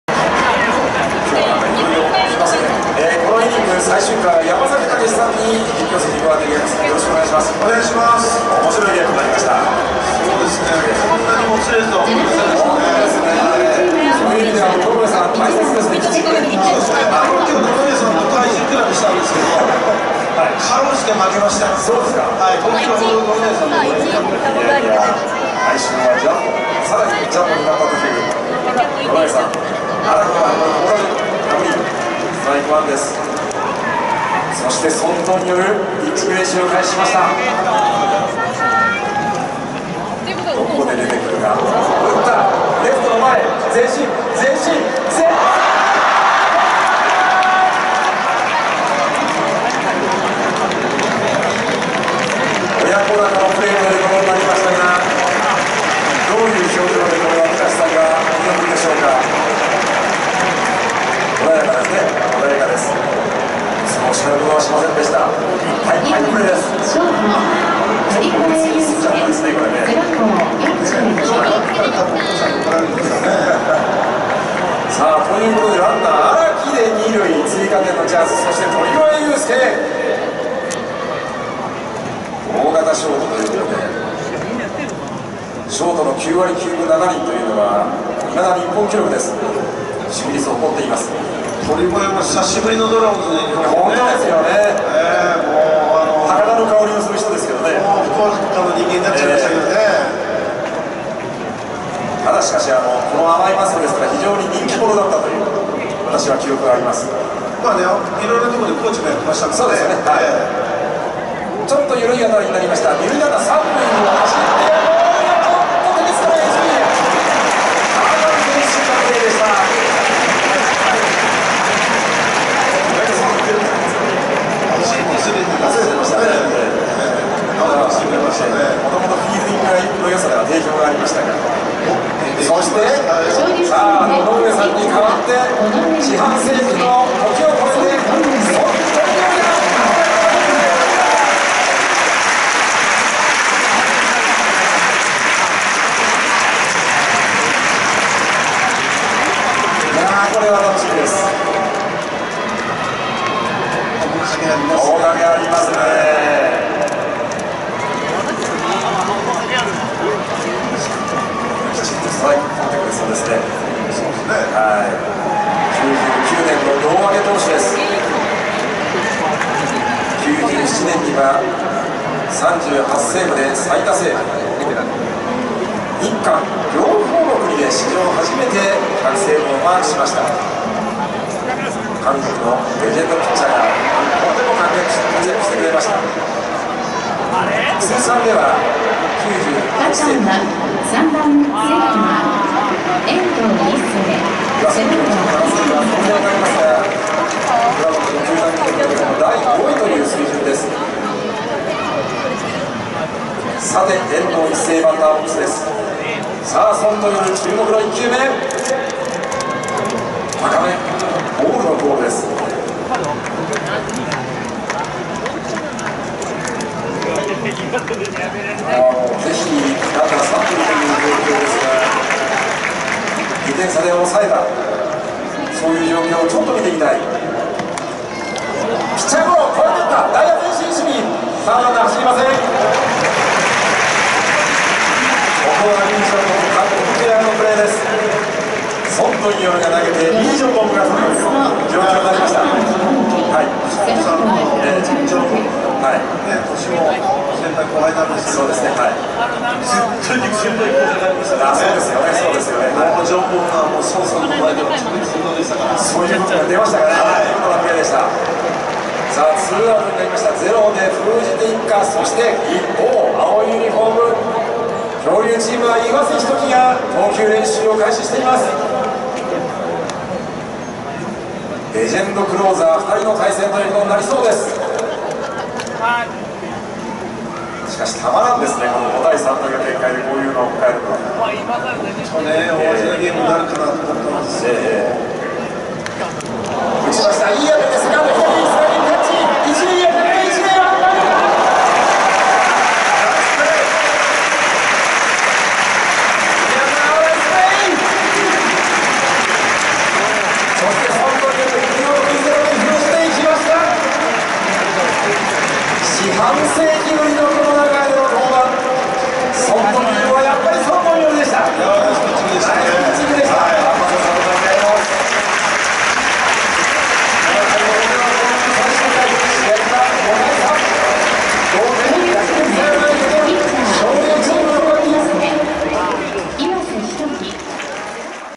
ドラゴンズ レジェンドOB紅白戦 2024 7回表！ ～ 堂上照 宣銅烈 武山真吾 荒木雅博 鳥越裕介 遠藤一星 前原博之 ～ サード川上憲伸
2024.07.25、昇竜チーム vs 強竜チーム＠バンテリンドームナゴヤ。

20240725